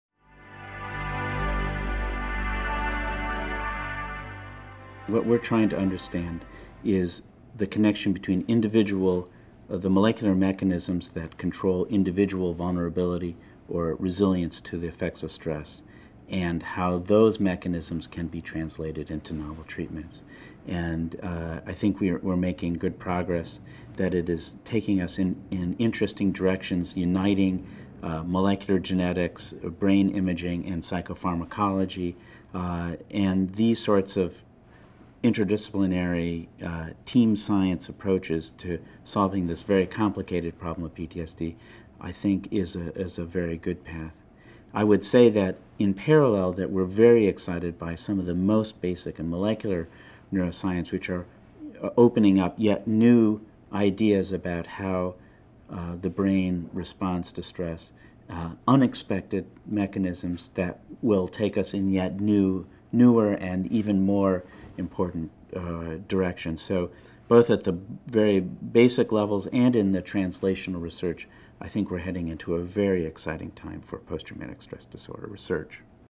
Dr. Krystal - Where Will Breakthroughs Come in the Next 10 Years PTSD?
Dr. John Krystal, Brain & Behavior Research Foundation Scientific Council Member, Editor of Biological Psychiatry, NARSAD Independent and Distinguished Investigator Grantee, Chair, Department of Psychiatry at Yale and Chief of Psychiatry at Yale-New Haven Hospital sat down with the Brain & Behavior Research Foundation (formerly NARSAD) to discuss post-traumatic stress disorder (PTSD) and the role that NARSAD Grants and the Brain & Behavior Research Foundation have played in mental health research. These interviews were taped at the 2009 Palm Beach Mental Health Research Symposium.